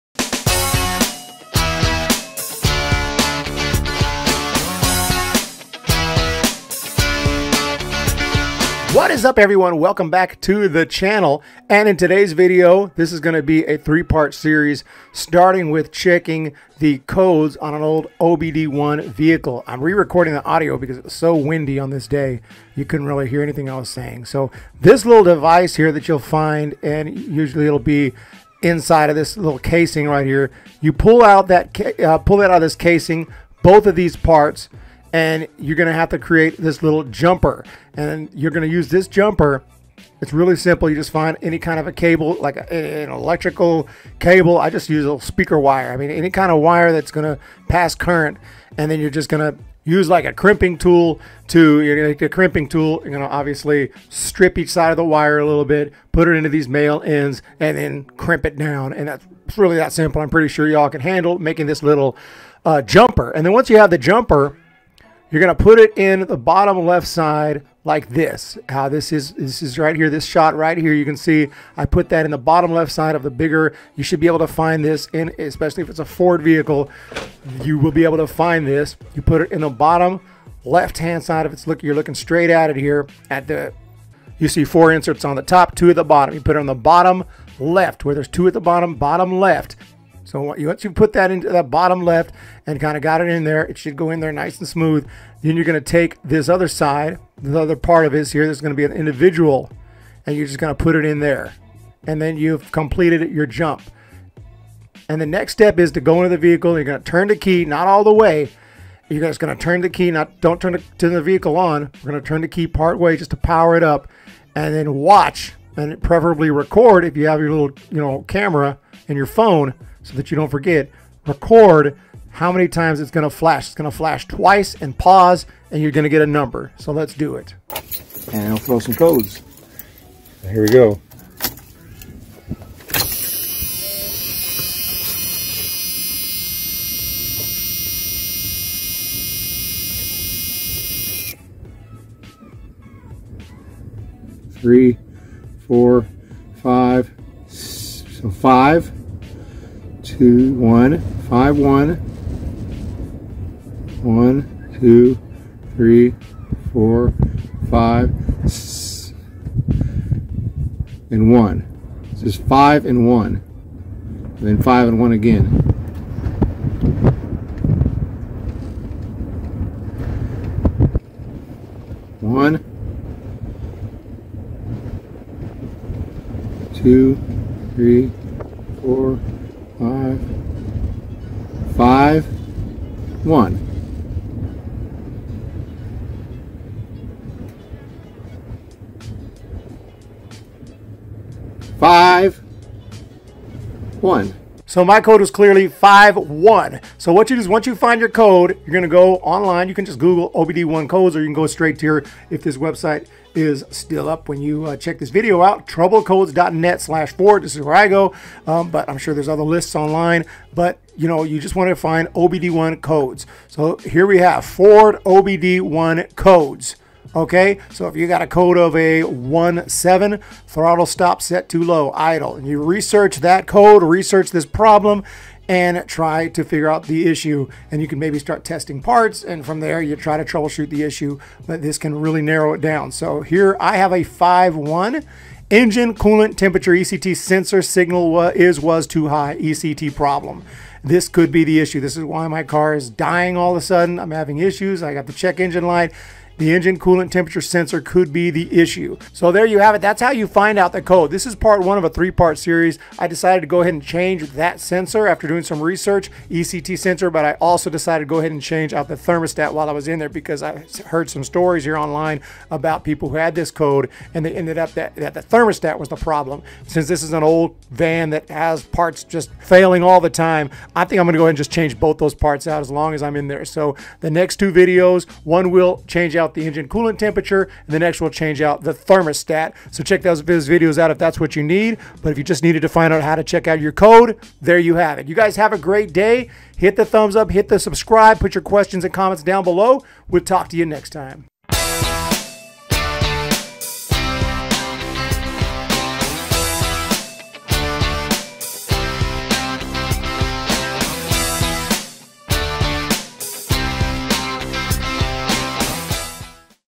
EASILY Read FORD OBD1 Check Engine Light CODES (1980-1995) WITHOUT A SCANNER... (i.e. Code 51)
In today's video, I check out the trouble code for a FORD 1988 E-150 Econoline Van.   I was able to extract the OBD1 code using a jumper wire with 2 male ends.  Follow the process in the video to get your trouble code.

If you have a Code 51 like I did, follow this series with the links below in order to change your ECT Sensor (Engine coolant Temperature sensor) and/or Thermostat.

FORD OBD-1 CODE 51: Engine Coolant Temperature (ECT) sensor signal is/was too high – ECT